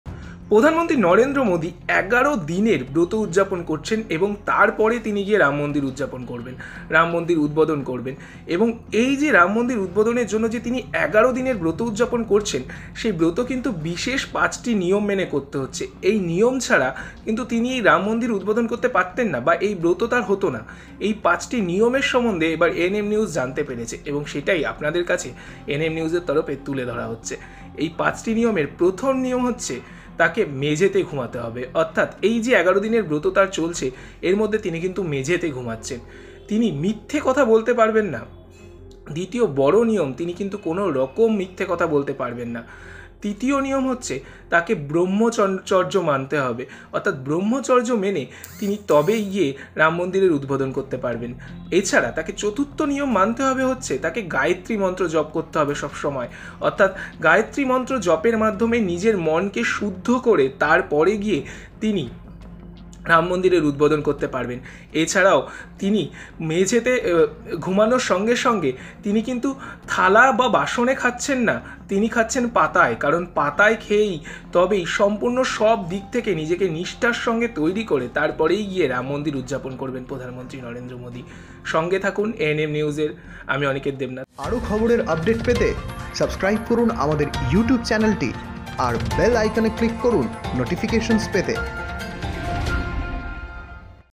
রাম মন্দিরের উদ্বোধন...মোদীকে মানতে হবে এই ৫ নিয়ম/ PM Modi's 11 day anusthan of Ram Mandir
নিজস্ব সংবাদদাতাঃ রাম মন্দির (Ram Mandir) নিয়ে কাউন্টডাউন শুরু হয়ে গিয়েছে। আর দুদিন পরেই রাম মন্দিরের দরজা সকলের জন্য খুলে যাবে। এদিকে রাম মন্দির 'প্রাণ প্রতিষ্ঠা'কে সামনে রেখে মোদীর ১১ দিনের 'অনুষ্ঠান' প্রসঙ্গে বড় দাবি করলেন শ্রী রাম জন্মভূমি তীর্থক্ষেত্রের প্রধান পুরোহিত আচার্য সত্যেন্দ্র দাস (Acharya Satyendra Das)। তিনি বলেন, "যে ব্যক্তি 'অনুষ্ঠান' করে তাকে মেঝেতে ঘুমাতে হয়, মিথ্যা কথা বলতে মানা থাকে, 'গায়েত্রী মন্ত্র'-এর মতো মন্ত্র জপ করতে হয়, পাতায় খেতে হয় এবং 'ব্রহ্মচর্য' অনুসরণ করতে হয় ।‘ মূলত দেশের প্রধানমন্ত্রী নরেন্দ্র মোদী ও অন্যান্য বিশিষ্টজনদের উপস্থিতিতে খুলবে রাম মন্দিরের দরজা।